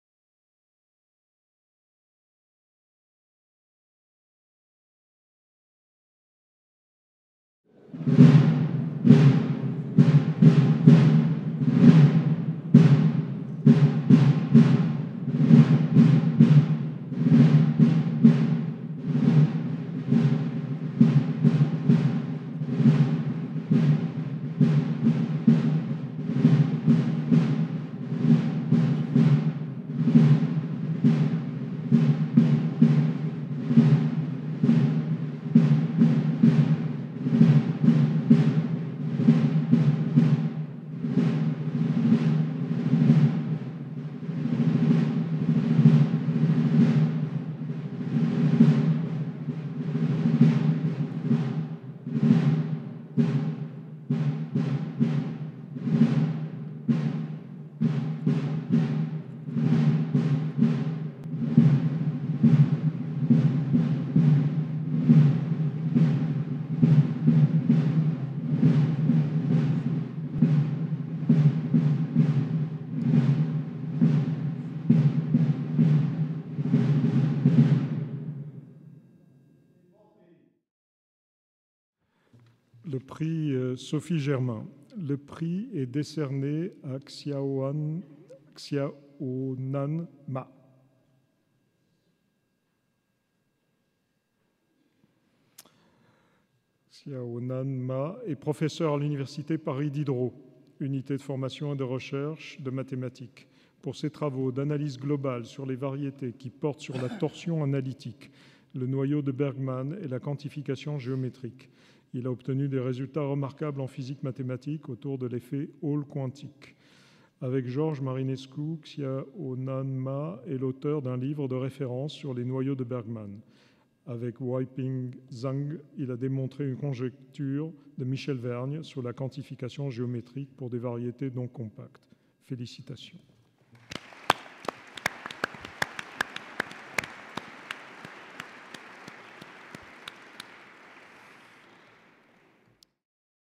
Lauréat 2017 du prix Sophie Germain : Xiaonan Ma
Séance solennelle de remise des prix 2017 de l'Académie des sciences Cérémonie sous la coupole de l'Institut de France le 21 novembre 2017

Le prix Sophie Germain 2017 est décerné à Xiaonan Ma, professeur à l’université Paris Diderot, unité de formation et de recherche (UFR) de mathématiques.
Le prix est décerné à Xiaonan Ma pour ses travaux d’analyse globale sur les variétés qui portent sur la torsion analytique, le noyau de Bergman et la quantification géométrique. Il a obtenu des résultats remarquables en physique mathématique autour de l’effet Hall quantique. Avec Georges Marinescu, Xiaonan Ma est l’auteur d’un livre de référence sur les noyaux de Bergman. Avec Weiping Zhang, il a démontré une conjecture de Michèle Vergne sur la quantification géométrique pour des variétés non compactes. Avec Marinescu et Zhang, il a obtenu des résultats importants sur le développement asymptotique du noyau de Bergman, ces résultats étant également démontrés dans un contexte équivariant. Enfin, ses travaux récents avec Klevtsov, Marinescu et Wiegman exhibent un lien surprenant entre le noyau de Bergman et l’effet Hall quantique.